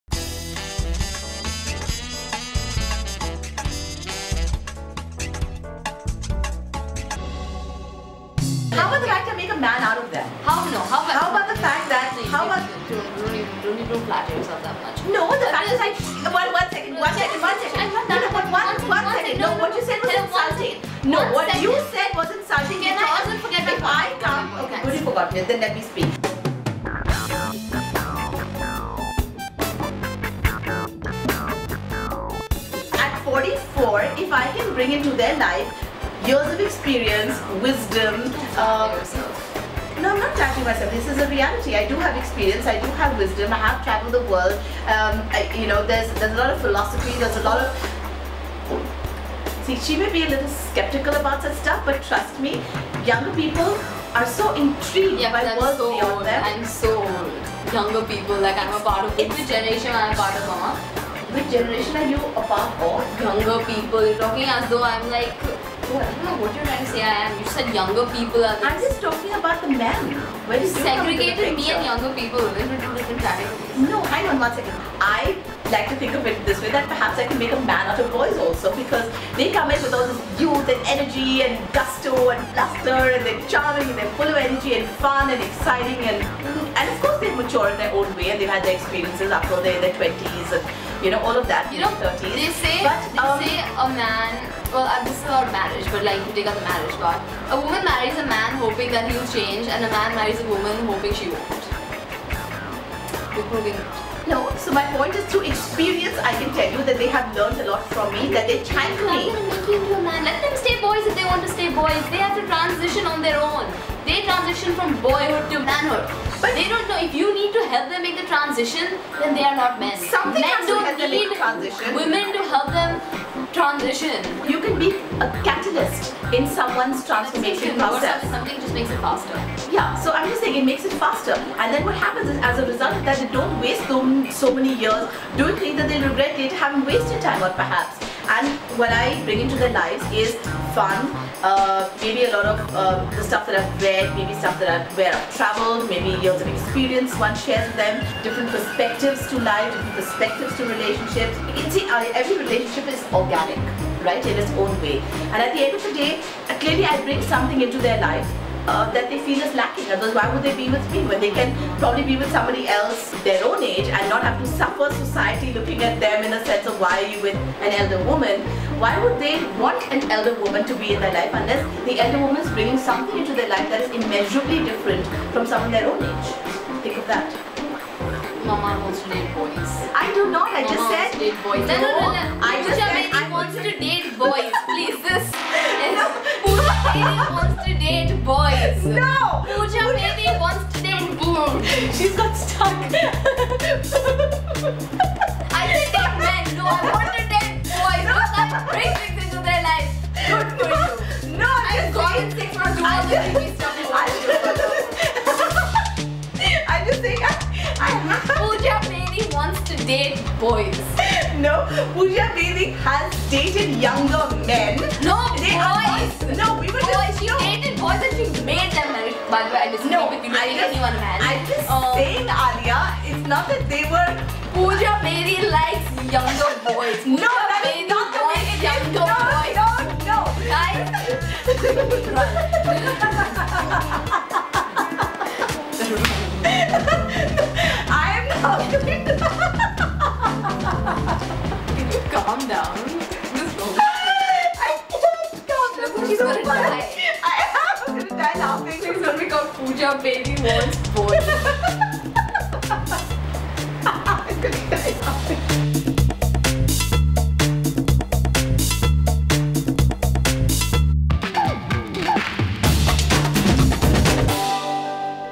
Pooja Bedi Wants To Date Boys!
In this video Pooja Bedi explains why she wants to date younger men. While Aaliia Eff supports all decision Pooja makes. Check out this episode to find out more!